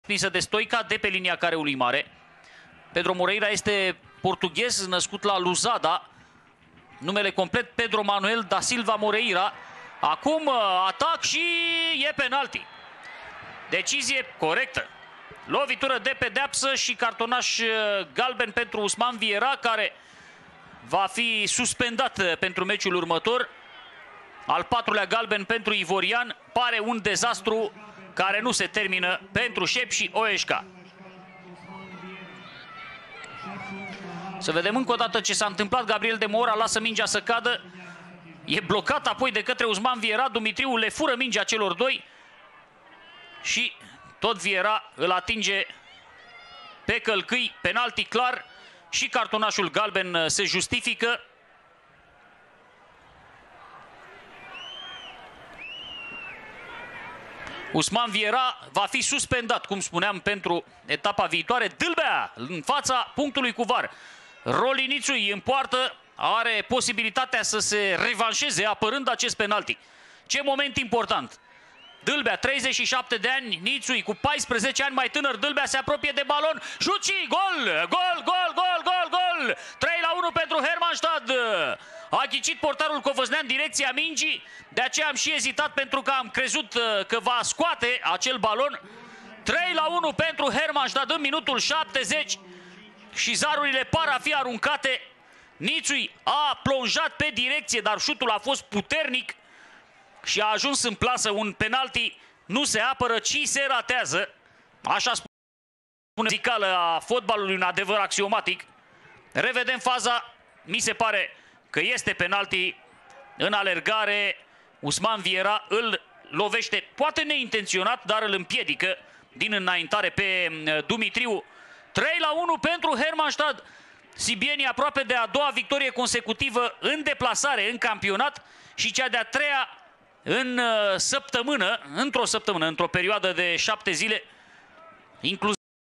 Sepsi - Hermannstadt 1- 3: Razvan Dalbea transforma lovitura de la 11m! - Liga 1 Etapa 14
Sepsi: Niczuly - Moura, Viera, Jovanovici, Sato - Velev, Vașvari - Nouvier, Tandia, Fl. Ștefan - Simonovski
Rezerve: Fejer, Drăghici, Ad. Rus, Hamed, Mensah, Karnitski
Antrenor: Eugen Neagoe

Hermannstadt: Căbuz - Company, C. Pîrvulescu, Dandea, I. Stoica, Tătar - Serediuc, Dâlbea, M. Dumitriu - Blănaru, Tsoumou
Rezerve: Niga, Antonov, Petrescu, Herghelegiu, Miranda, Nkololo